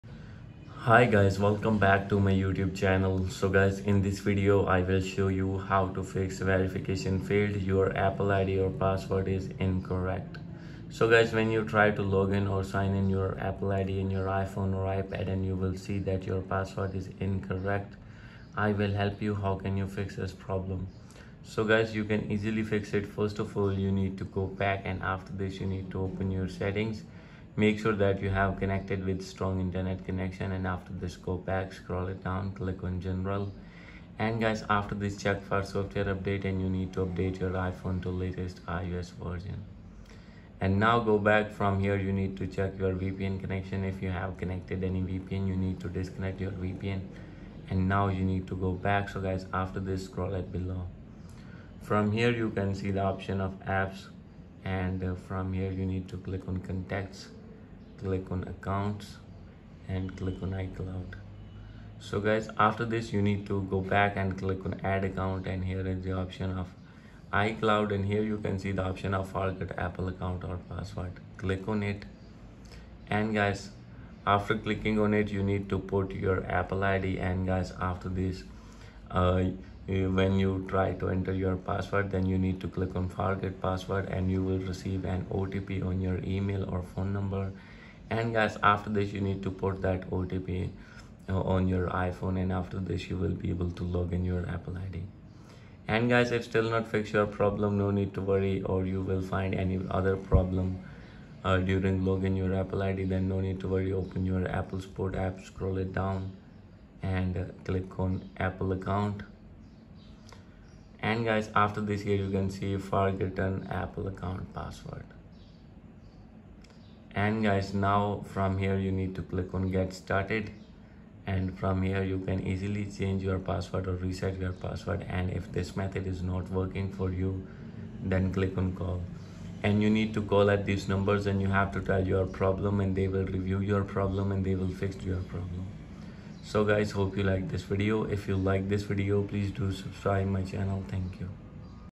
Apple ID Login Problem Solved! | "Your Apple ID or Password Is Incorrect" Fix 2025
🔧 Are you facing the “Your Apple ID or Password is Incorrect” error after the 2025 iOS update? Don’t worry – in this video, UnlockiOS Tech shows you the latest working methods to fix this frustrating Apple ID login issue on your iPhone, iPad, or iPod Touch.

📱 Whether you’re on iOS 17, iOS 18 Beta, or just updated your device, this quick 2-minute tutorial will help you solve the problem easily.

💡 In this video:
• Real reasons behind the Apple ID login error
• Updated fix for 2025 Apple ID issues
• Bonus tips to prevent future sign-in errors